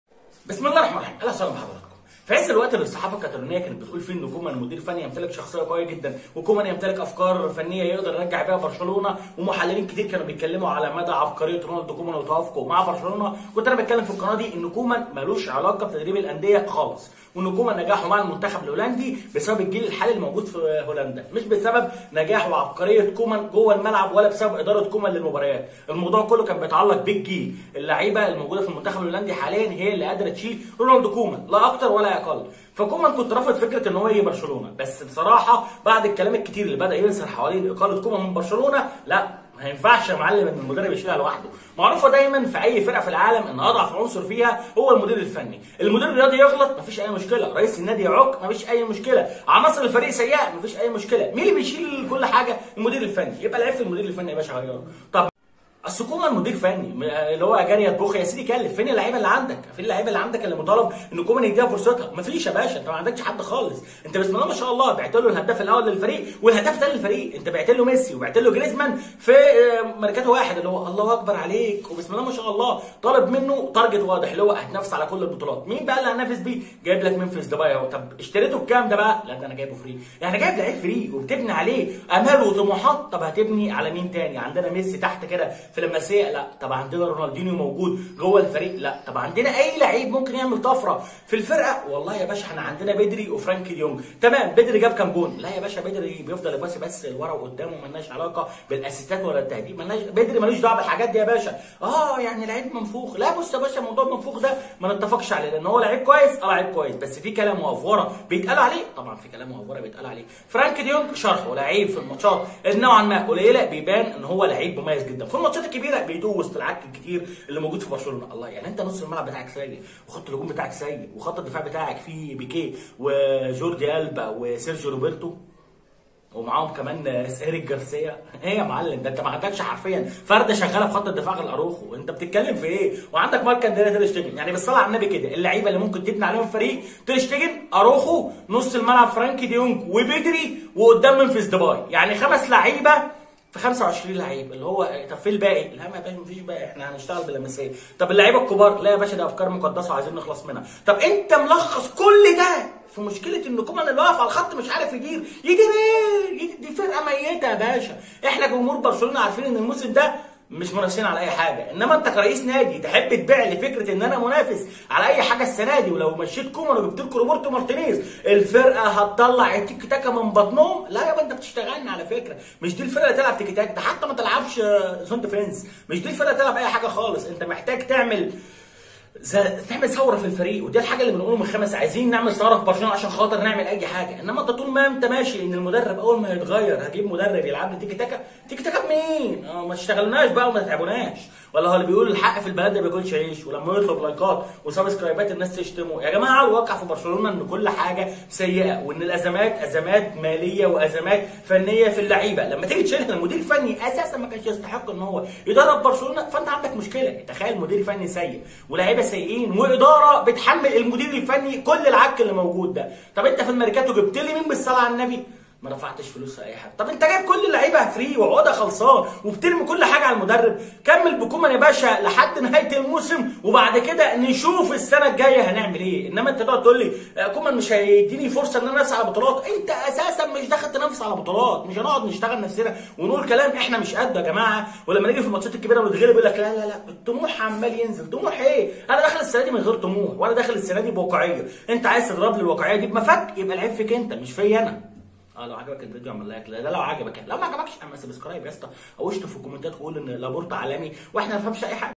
ماذا بعد اقاله رونالد كومان من برشلونه هل برشلونه منافس علي البطولات فعلا والمشكله في كومان🤔🤔
اول ما تم تعيين رونالد كومان مدرب جديد لفريق برشلونه وانا بقول انو ملوش فيها بس بعد خساره برشلونه من بايرن ميونخ في دوري ابطال اوروبا والكلام كتر جدا حوالين اقاله_رونالد_كومان من برشلونه ومين البديل ليه والكلام هنا عن المدرب الابرز لخلافه كومان وهو روبيرتو مارتنيز مدرب المنتخب البلجيكي وهنا محتاجين نتكلم شويه عن برشلونه وهل كومان بس هو المذنب الوحيد في برشلونه